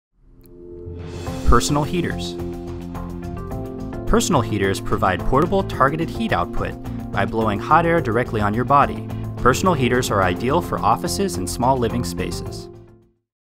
What Are Personal Heaters-2?
Personal heaters provide portable, targeted heat output by blowing hot air directly on your body that is ideal for offices and small living space